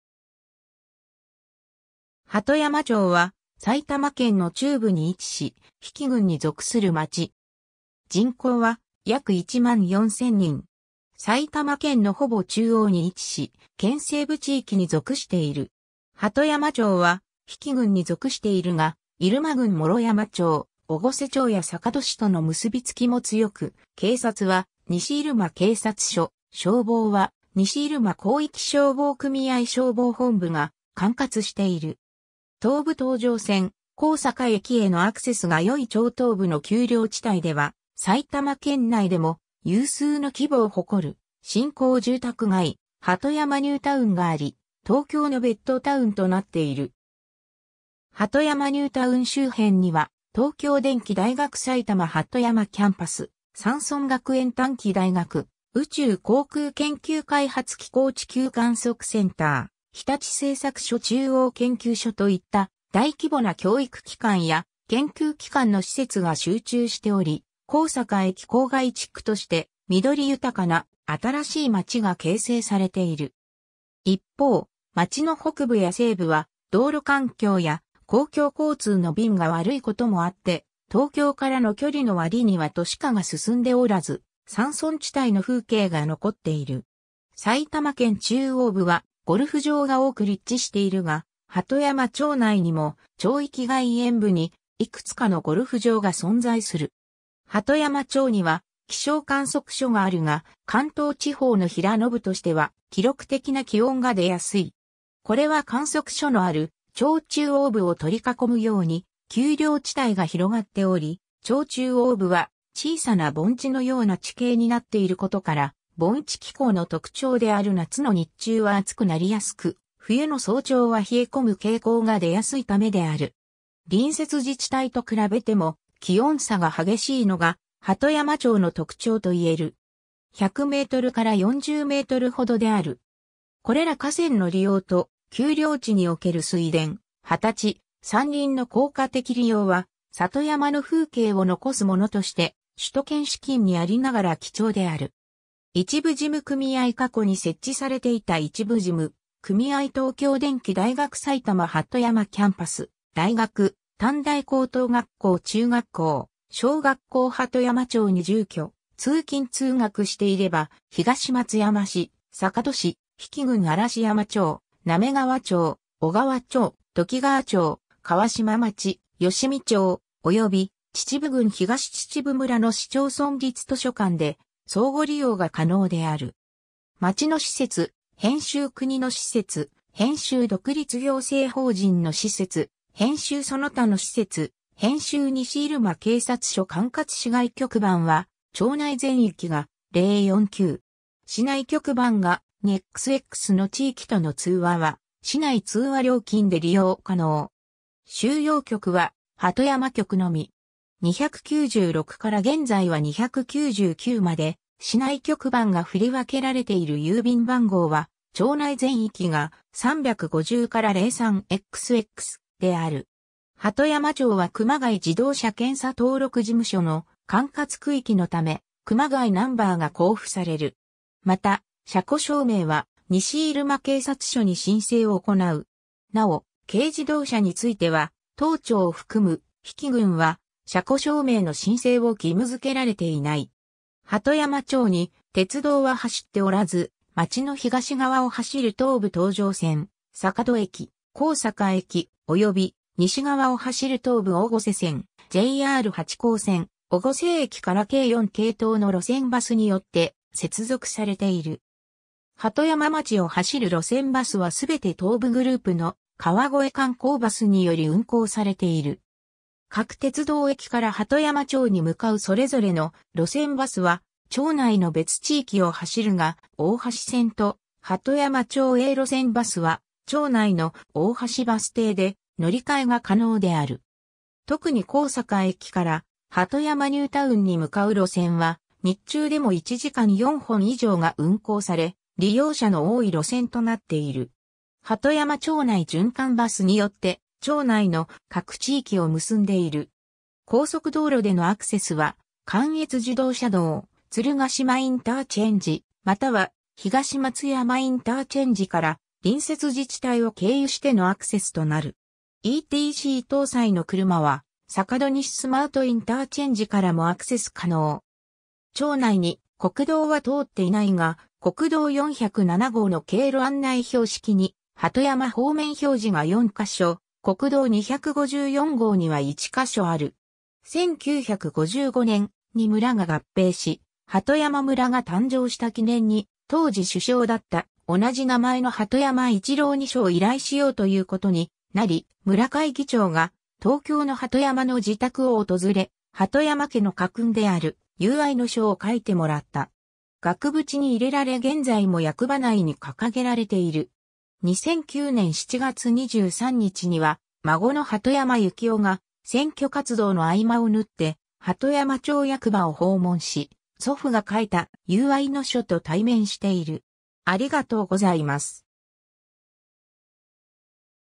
鳩山町
鳩山町（はとやままち）は、埼玉県の中部に位置し、比企郡に属する町。
 人口は約1万4千人。
 埼玉県のほぼ中央に位置し、県西部地域に属している。
 鳩山町は比企郡に属しているが、入間郡毛呂山町・越生町や坂戸市との結びつきも強く、警察は西入間警察署（旧:越生警察署）、消防は西入間広域消防組合消防本部が管轄している。
東武東上線・高坂駅（東松山市）へのアクセスが良い町東部の丘陵地帯では埼玉県内でも有数の規模を誇る新興住宅街「鳩山ニュータウン」があり、東京のベッドタウンとなっている。
鳩山ニュータウン周辺には「東京電機大学埼玉鳩山キャンパス」、「山村学園短期大学」、「宇宙航空研究開発機構地球観測センター」、「日立製作所中央研究所（鳩山地区）」といった大規模な教育機関や研究機関の施設が集中しており、高坂駅郊外地区として緑豊かな新しい街が形成されている。
 一方、町の北部や西部は道路環境や公共交通の便が悪いこともあって東京からの距離の割には都市化が進んでおらず、山村地帯の風景が残っている。
 埼玉県中央部はゴルフ場が多く立地しているが、鳩山町内にも町域外縁部にいくつかのゴルフ場が存在する。
 鳩山町には気象観測所（アメダス）があるが、関東地方の平野部としては記録的な気温が出やすい。
これは観測所のある町中央部を取り囲むように丘陵地帯が広がっており、町中央部は小さな盆地のような地形になっていることから、盆地気候の特徴である夏の日中は暑くなりやすく、冬の早朝は冷え込む傾向が出やすいためである（1月の平均最低気温：-3.9 ℃、氷点下10度近くを記録することがあり、冬の最低気温は秩父市とほとんど同じである）。
隣接自治体と比べても気温差が激しいのが鳩山町の特徴と言える。
 100m～40mほどである。
 これら河川の利用と丘陵地における水田、畑地、山林の効果的利用は「里山の風景」を残すものとして首都圏至近にありながら貴重である。
 一部事務組合 過去に設置されていた一部事務組合 　（ゆうちょ銀行については下記郵政の項目を参照のこと） 　（かつては小川信用金庫鳩山支店、みずほ銀行鳩山出張所が存在したがそれぞれ撤退した） 東京電機大学埼玉鳩山キャンパス 大学・短大 高等学校 中学校 小学校 鳩山町に住居・通勤通学していれば、東松山市、坂戸市、比企郡嵐山町、滑川町、小川町、ときがわ町、川島町、吉見町、及び秩父郡東秩父村の市町村立図書館で相互利用が可能である。
 町の施設[編集] 国の施設[編集] 独立行政法人の施設[編集] その他の施設[編集] 西入間警察署（坂戸市）管轄 市外局番は町内全域が「049」。
市内局番が「2XX」の地域との通話は市内通話料金で利用可能（川越MA）。
収容局は鳩山局のみ。
 296から現在は299まで市内局番が振り分けられている 郵便番号は町内全域が「350-03xx」（鳩山郵便局）である。
 鳩山町は熊谷自動車検査登録事務所の管轄区域のため、「熊谷」ナンバーが交付される。
 また、車庫証明は西入間警察署に申請を行う。
なお、軽自動車については、当町を含む「比企郡」は車庫証明の申請を義務付けられていない。
 鳩山町に鉄道は走っておらず、町の東側を走る東武東上線「坂戸駅」「高坂駅」、および西側を走る東武越生線・JR八高線「越生駅」から計4系統の路線バスによって接続されている。
 鳩山町を走る路線バスはすべて東武グループの川越観光バスにより運行されている。
各鉄道駅から鳩山町に向かうそれぞれの路線バスは町内の別地域を走るが、大橋線と鳩山町営路線バスは町内の「大橋」バス停で乗り換えが可能である。
特に高坂駅から鳩山ニュータウンに向かう路線は日中でも1時間4本以上が運行され、利用者の多い路線となっている。
 「鳩山町内循環バス」によって町内の各地域を結んでいる。
 高速道路でのアクセスは、関越自動車道・鶴ヶ島インターチェンジ（鶴ヶ島市）、または東松山インターチェンジ（東松山市）から隣接自治体を経由してのアクセスとなる。
ETC搭載の車は坂戸西スマートインターチェンジ（坂戸市）からもアクセス可能。
 町内に国道は通っていないが、国道407号の経路案内標識に「鳩山」方面表示が4カ所（東松山市内の毛塚、宮鼻、高坂神社東、上野本南の各交差点）、国道254号には1カ所（東松山市内の上野本西交差点）ある。
 1955年、2村が合併し鳩山村が誕生した記念に、当時首相だった同じ名前の鳩山一郎に書を依頼しようということになり、村会議長が東京の鳩山の自宅を訪れ...